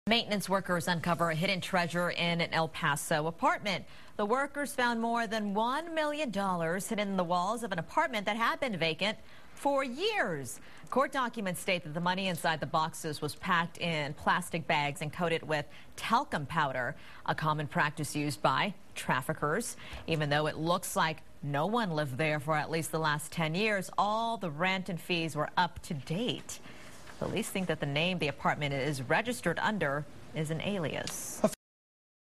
El Paso drug cash
The money El Paso maintenance workers found in a condo while doing repairs is believed to be drug money.